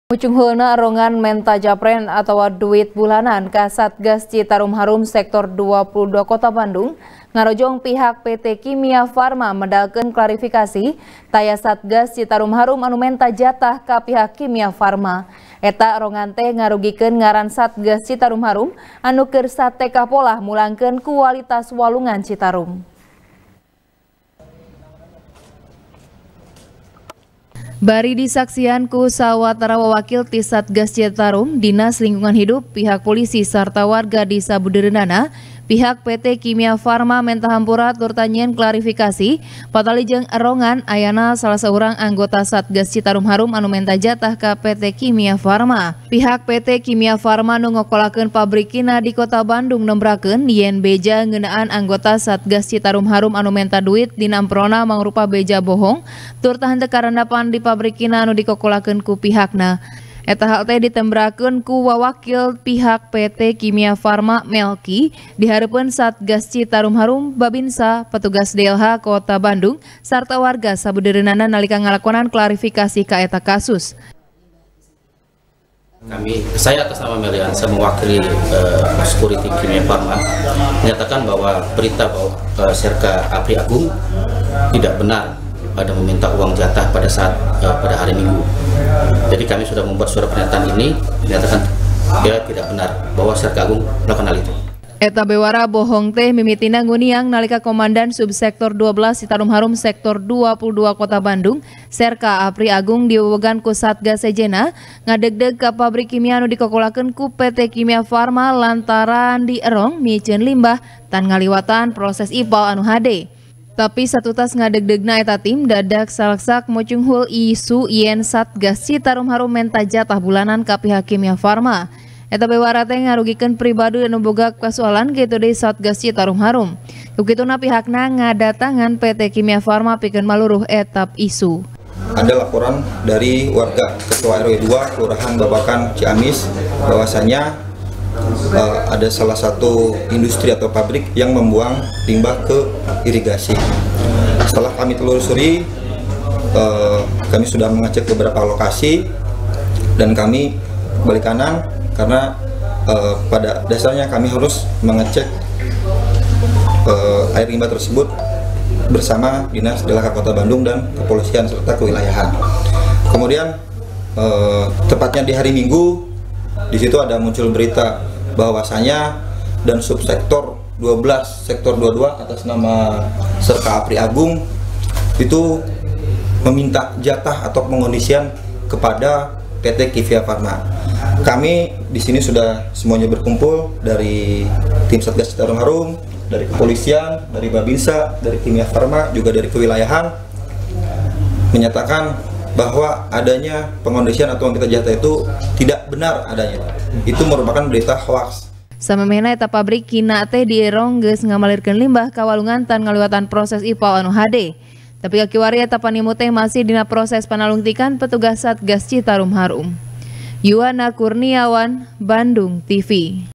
Nuding Japrem Citarum Harum, PT. Kimia Farma Klarifikasi | TP | BANDUNGTV
TP | BANDUNGTV | ELBY NOPITRI | Mucunghulna Erongan Menta Japrem Atawa Duit Bulanan Ka Satgas Citarum Harum Sektor Dua Puluh Dua Kota Bandung, Ngarojong Pihak PT Kimia Farma Medalkeun Klarifikasi, Taya Satgas Citarum Harum Anu Menta Jatah Ka Pihak Kimia Farma. Eta Erongan Teh Ngarugikeun Ngaran Satgas Citarum Harum Anu Keur Satekah Polah Mulangkeun Kualitas Walungan Citarum.